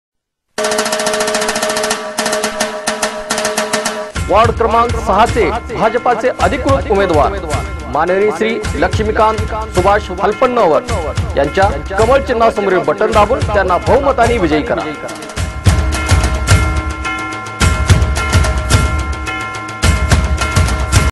वार्डक्रमांक 6चे भाजपचे अिधकृत उमेदवार मा.श्री.लक्ष्मीकांत सुभाष हालप्पनवर
वार्डक्रमांक 6चे भाजपचे अिधकृत उमेदवार मा.श्री.लक्ष्मीकांत सुभाष हालप्पनवर यांच्या कमळ िचन्हा समोरील बटन दाबुन त्यांना बहुमतांनी िवजयी करा